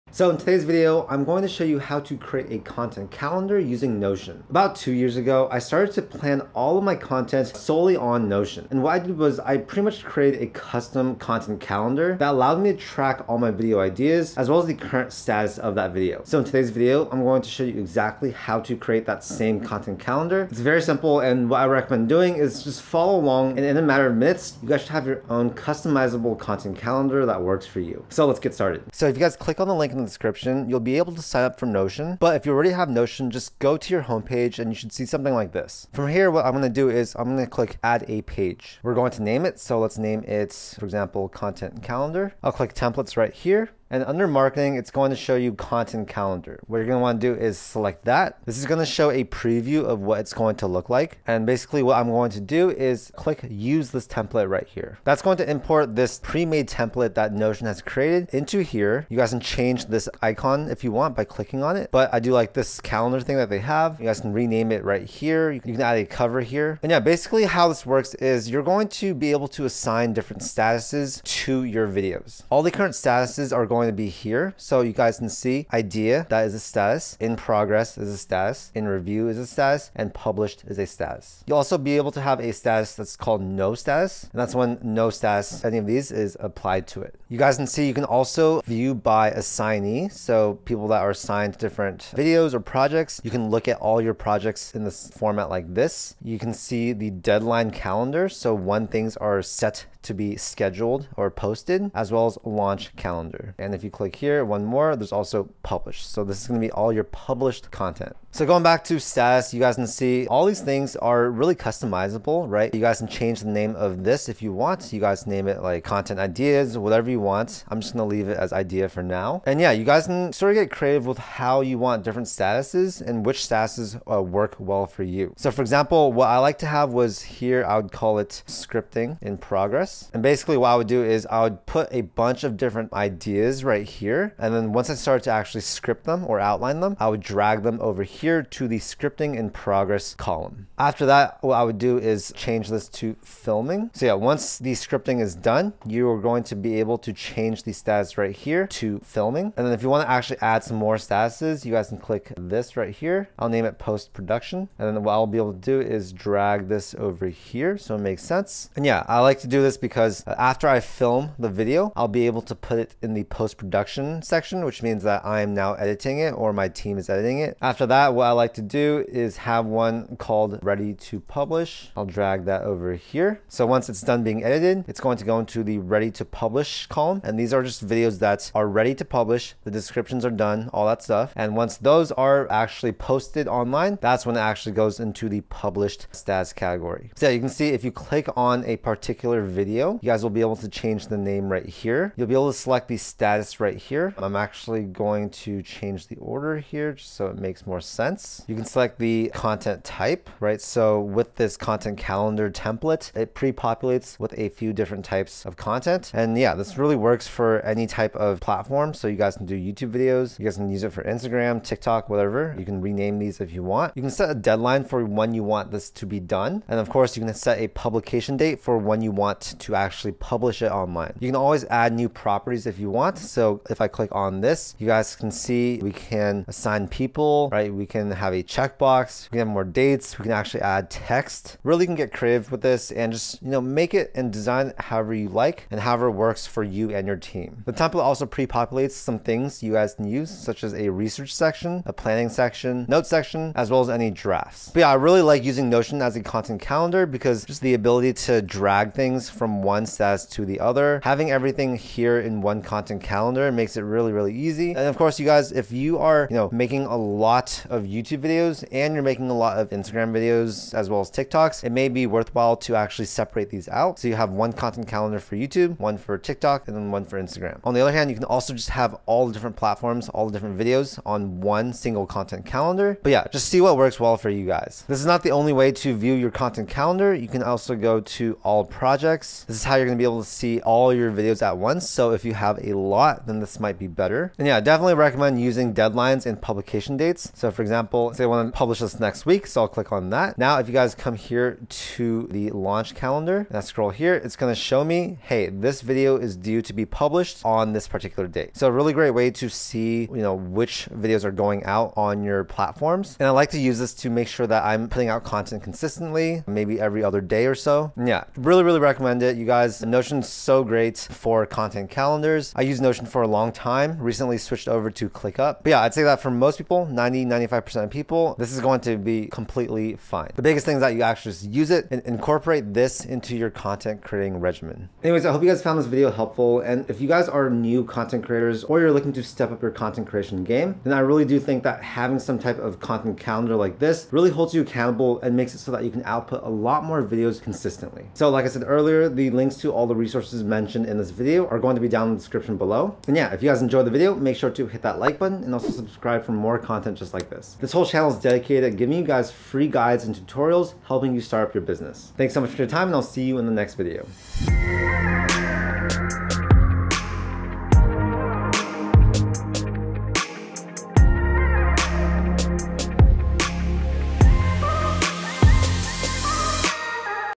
Creating Content Calendars in Notion: A Guide to Organizing & Producing Content Online
In this video, I'll be going over my favorite productivity app Notion, and how to create a content calendar on the platform. About 2 years ago, I started planning out my content on Notion, and it's been really useful in helping me keep track of all my video ideas, as well as the current status of the videos.

Our favorite business must-haves:
💳 Best business credit cards:

If you're looking for an app to organize your life and allow you to make content more efficiently, this is it! I always recommend Notion for content creators because it allows you to plan ahead and batch film videos which is super efficient for people that do content as a side hustle! Be sure to watch through until the end of the video because I'll be showing you guys how you can set up your own customizable content calendar within minutes.

Our goal is to help entrepreneurs with business formation, side hustles, and making money!

Have a wonderful rest of your day!

-Charlie

Timeline:
0:00 - Intro
0:29 - Notion Website Walkthrough
5:25 - Conclusion